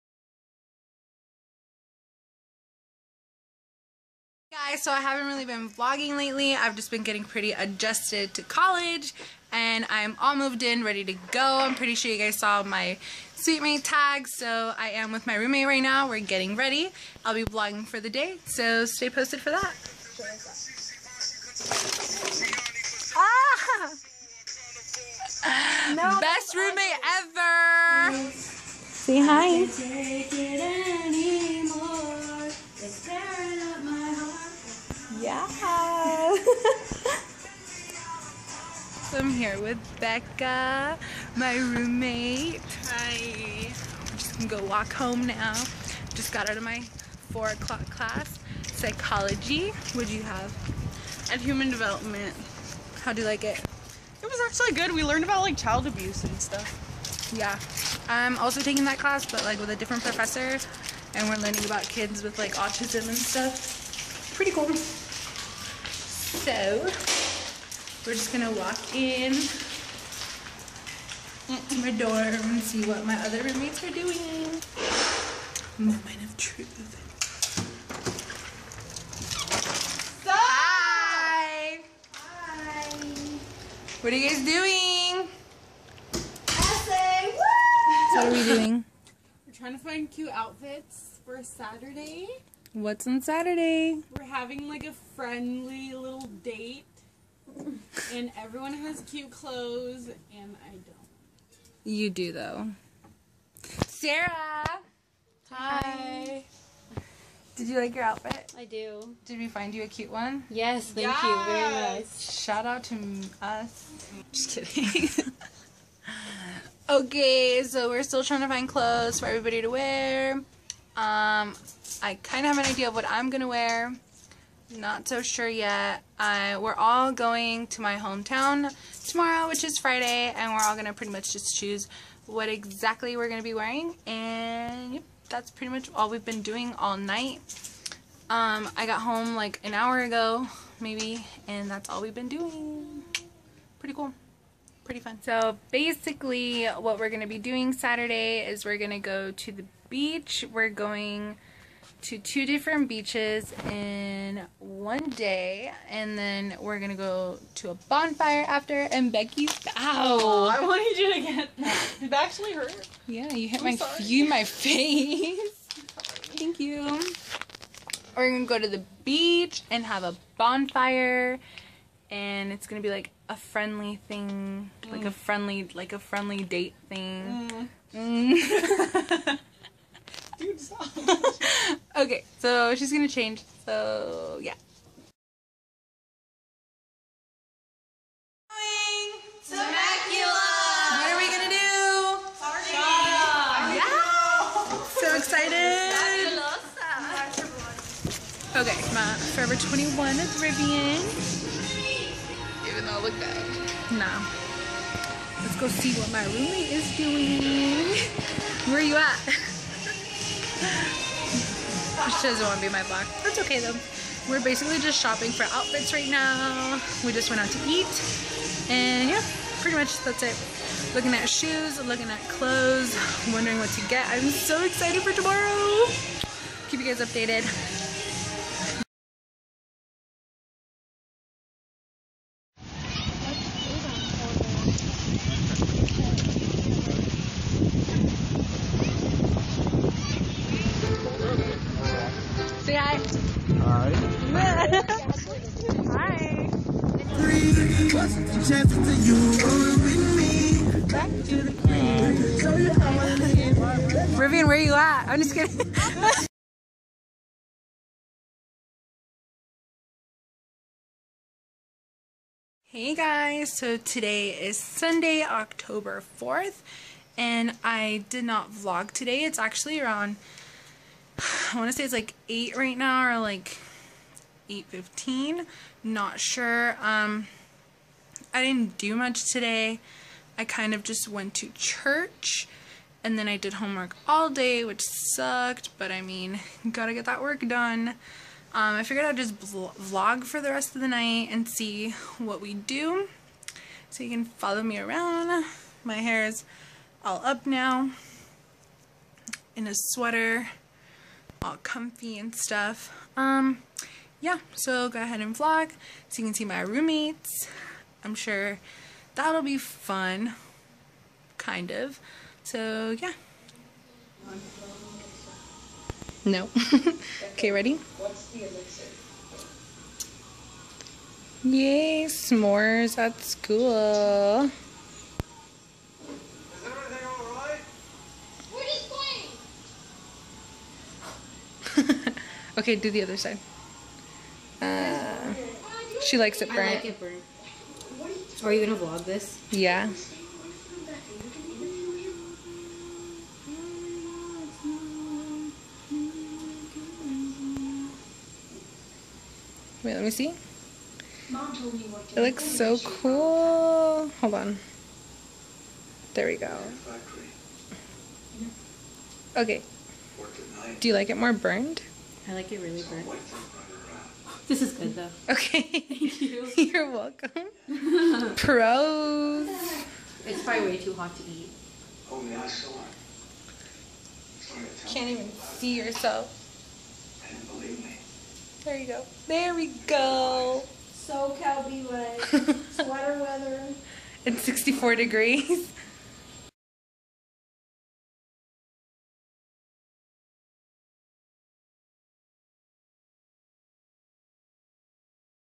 College Vlog: Shopping, Bonfire & Burning Marshmallows
Hi Guys!
I hope you enjoy the vlog where you get to see what my college experience has been so far!